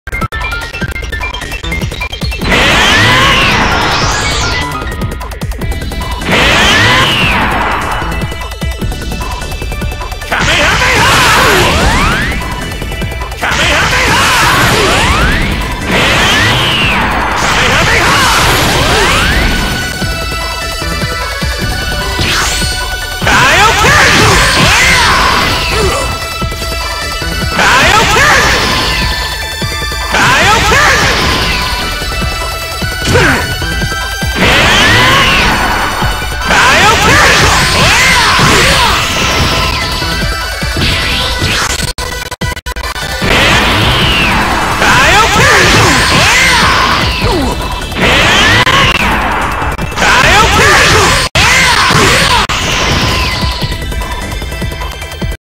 DJMouF Goku Back to this guy Finally
It's been a while since I've done anything with this guy here. I updating a lot on him so it'll be a while til he's released. Kaioken Attack needed a huge update and I like it much better. I like the lvl1/2 kame blasts. He still needs other super attacks which I haven't really decided yet. But I'll get them soon. Spirit Bomb and Meteor Strike will be his two Lvl3 Supers.